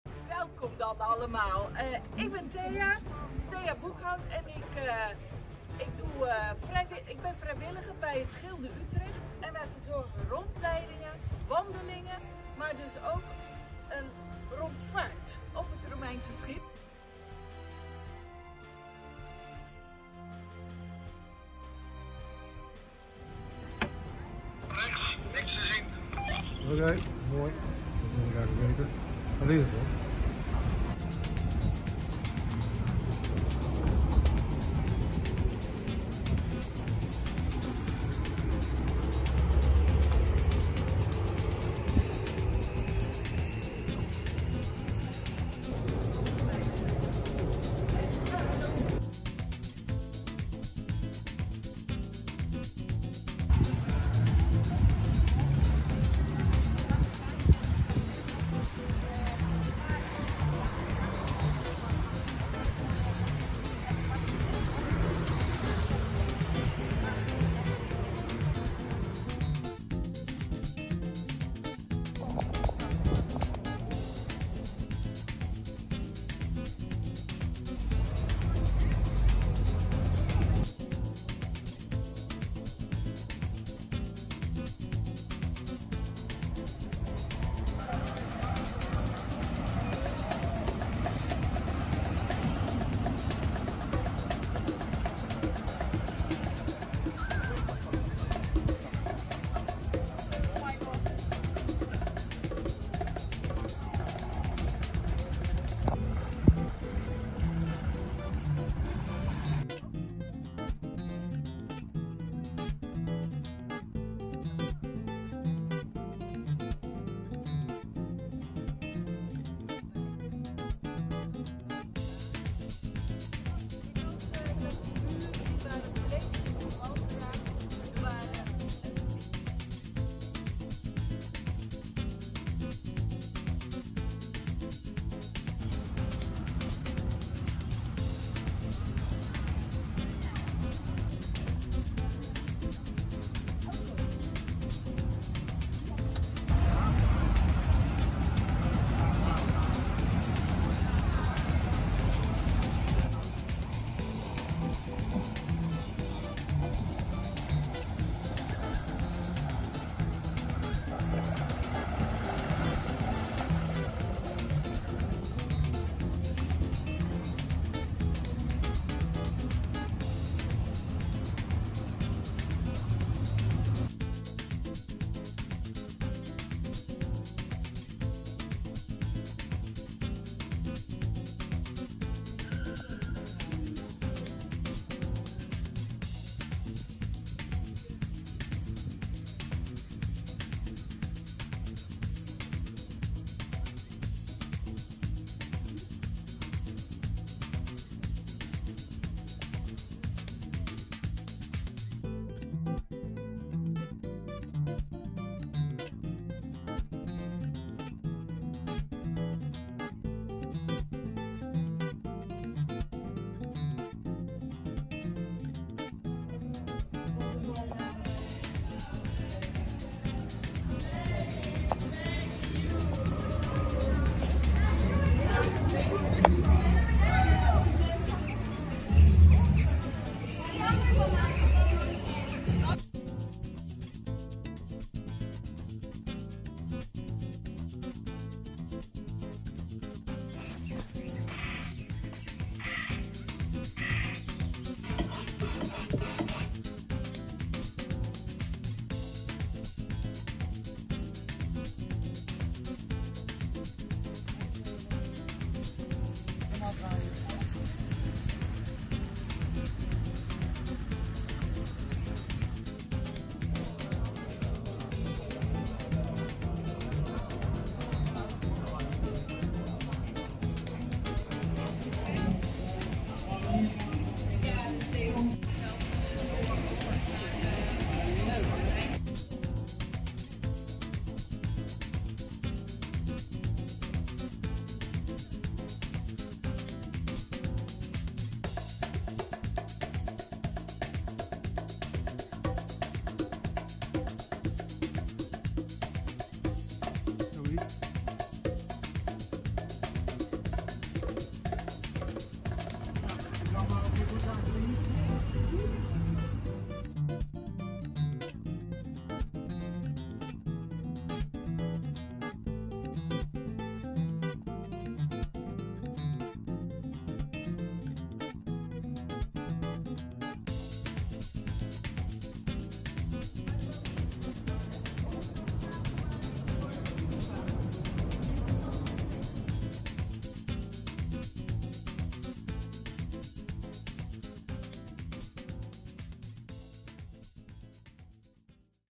MET DE ROMEINEN DE SINGEL OP
Als het schip DeMeern1 over de singel vaart, trekt het heel wat bekijks. Niet verwonderlijk, want het ziet er bijzonder uit. Het is een open boot, smal. Maar vooral lang, heel lang: 24,6 meter.
Het is een replica van een Romeinse rijnaak, die in 1997 in De Meern werd ontdekt en waarvan het wrak in 2003 werd opgegraven. Het oorspronkelijke schip dat dateert uit de periode 150-190 na Chr. is gestrand in een bocht van de toenmalige Rijn. De aak werd voortgetrokken en had een klein zeil ter ondersteuning.
Het e-schip is door Bouwloods nagemaakt en kreeg de naam Per Mare ad Laurium. De replica voer eerst in Woerden, maar vaart nu na een opknapbeurt op de museumwerf in Vreeswijk op aanvraag voor kleine gezelschappen een rondje singel (pardon: stadsbuitengracht).
‘Het is anders varen’, vertelt Bastiaan van Achterbergh. Normaal is hij schipper bij rederij Schuttevaer. ‘De lengte maakt het anders.’ Voorop de fluisterboot zit een matroos, die om een hoekje kijkt en via een portofoon Van Achterbergh waarschuwt voor tegemoetvarende waterfietsen, kano’s, sloepen en jachten.
De vaartocht begint op de Oosterkade en gaat tegen de klok in. Vanaf het water zijn de rietkragen op de kade des te indrukwekkender. Maar ook het onderaanzicht van Hoog Catharijne intrigeert en het is genieten van de resten van kasteel Vredenburg net als de begroeiing van het laatst herstelde stuk singel.

tekst: Ineke Inklaar
video: Michael Kooren